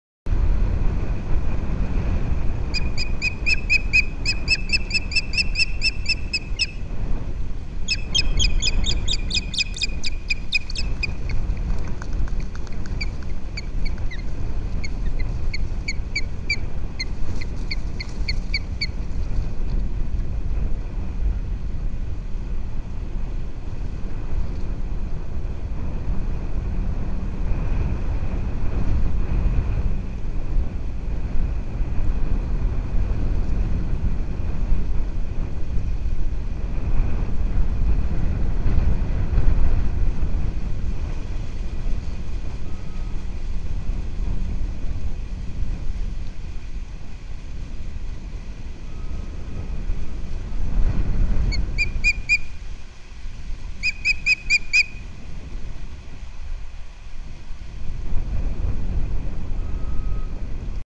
Captiva Island Osprey Cam, Florida, USA, January 19, 2023 (2)
And working again.🤣

Courtesy of Window To Wildlife.